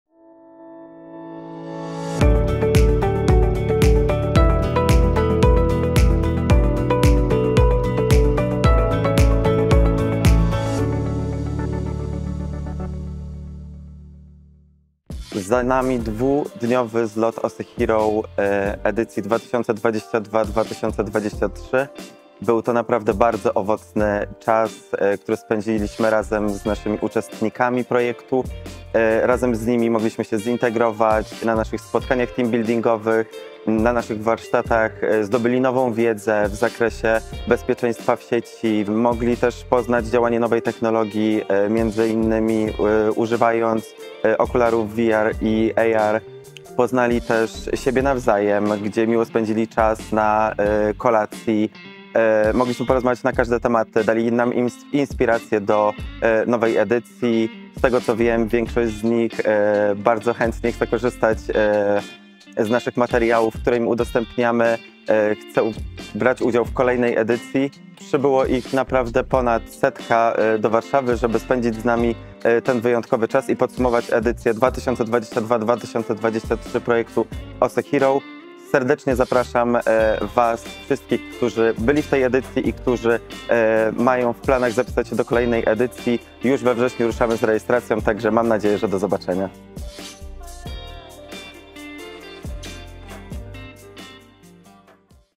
Zlot OSEhero 2023 | Wywiad z Marcinem Tomczykiem
Dwa dni Zlotu OSEhero 2023 podsumował Marcin Tomczyk koordynator Projektu.